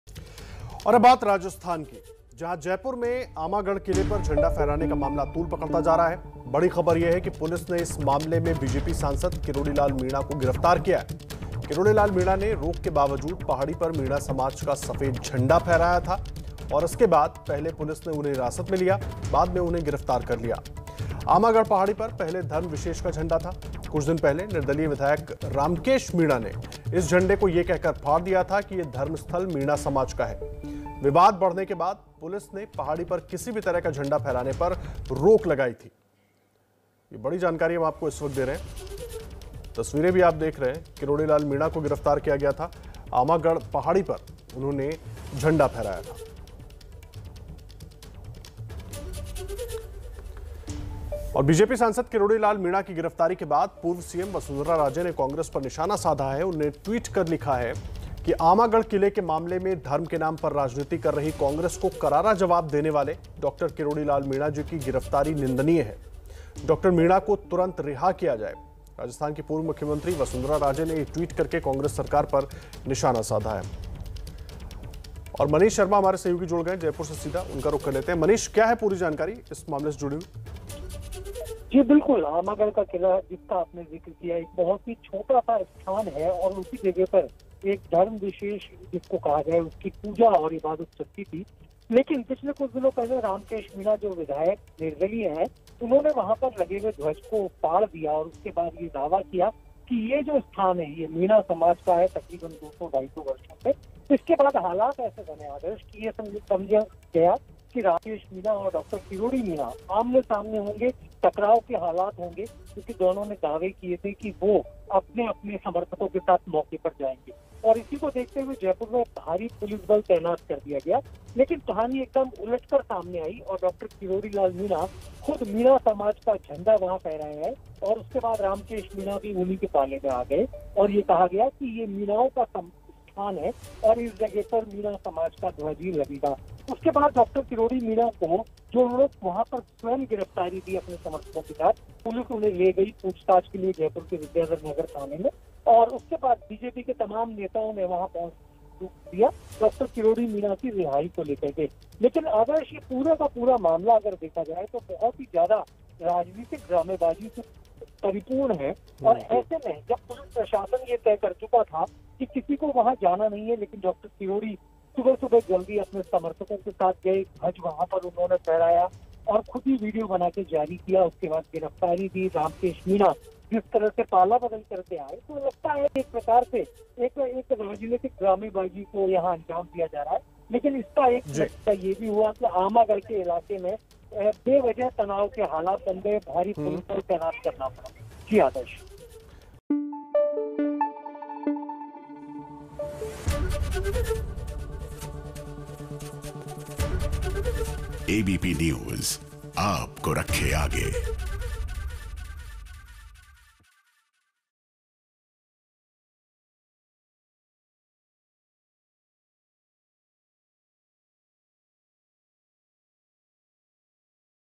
रोक के बावजूद झंडा फहराने पर हुई BJP सांसद किरोड़ी लाल मीणा की गिरफ्तारी | ABP News Hindi
जयपुर के आमागढ़ किले पर झंडा फहराने का मामला तूल पकड़ता जा रहा है. विवाद ये है की पुलिस के रोक के बावजूद आमागढ़ पहाड़ी पर किरोड़ी लाल मीणा ने झंडा फहराया. इस वजह से उन्हें गिरफ्तार किया गया है